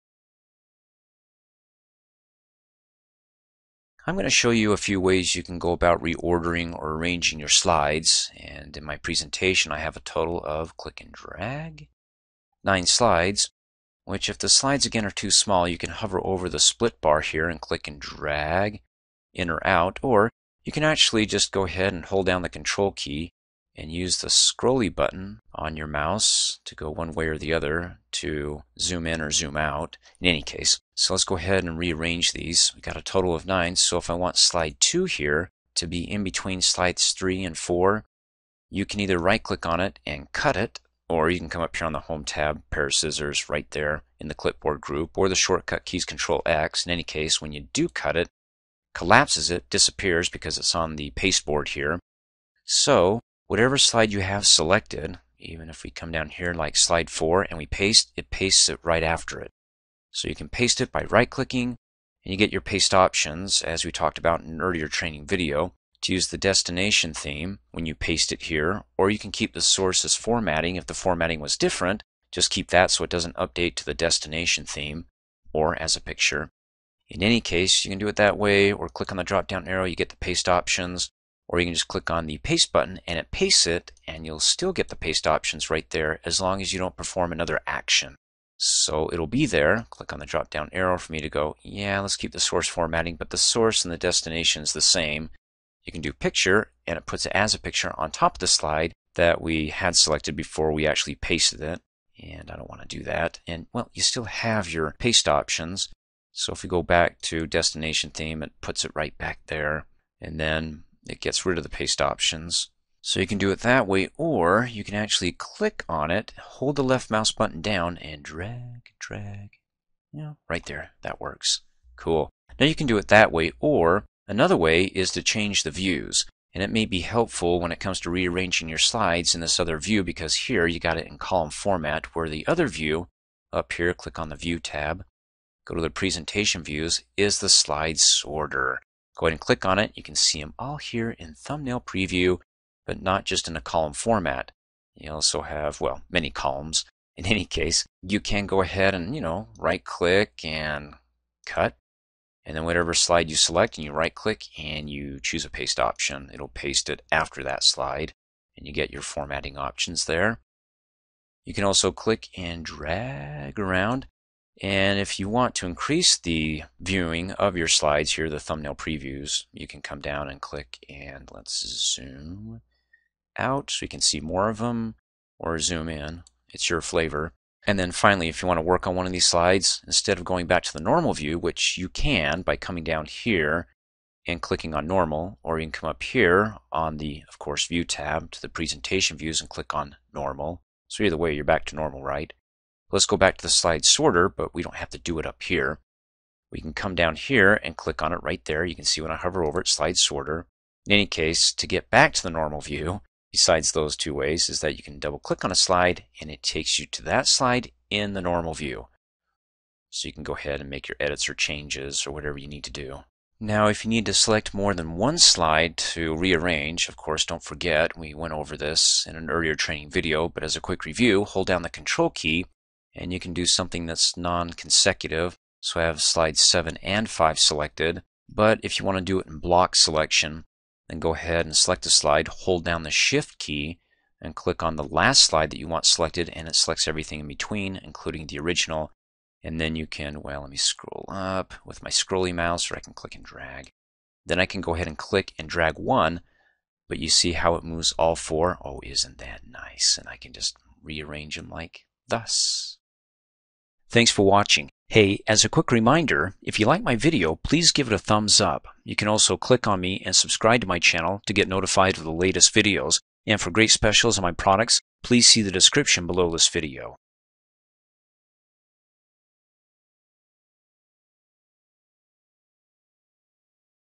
Microsoft PowerPoint 2016: Arrange and Reorder Slides Fast!
Microsoft PowerPoint 2016 tutorial on how to arrange, or reorder your slides around in your presentation quickly using the Slide Sorter view.